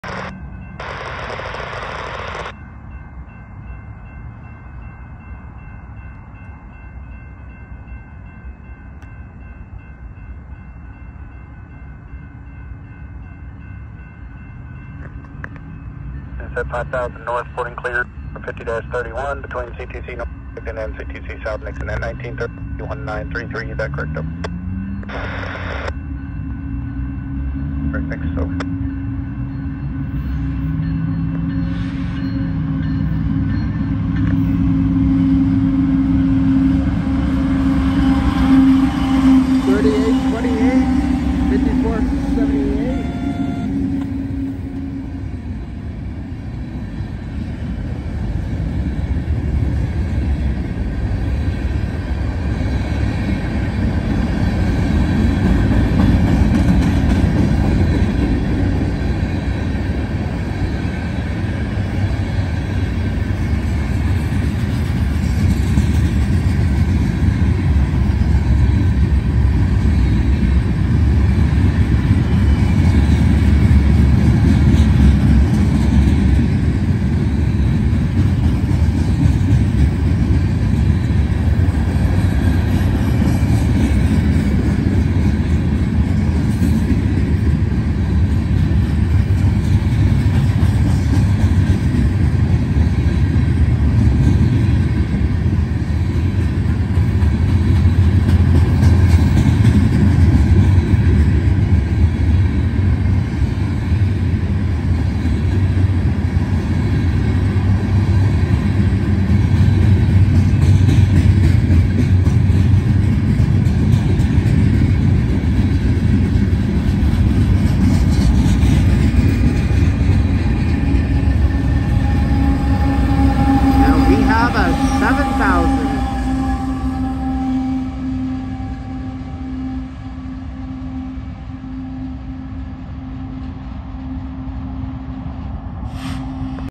BNSF #3828 Lead’s The SB U-CHELAJ9-15A With Single DP Power Unit 11/17/23.
So The Last Train That We Had Was SB BNSF U-DENLAJ With Three Total BNSF Unit’s To Pueblo Then To Trinidad For The Crew & Also To La Junta, CO With Single BNSF ET44C4 Lead’s This Great Ballast Down The Pike Peak SUB Hope You Enjoy This Amazing Video.

BNSF SB U-CHELAJ9-15A At 19:32pm
#3828 GEVO ET44C4
#5478 GE C44-9W
#7000 GE ES44C4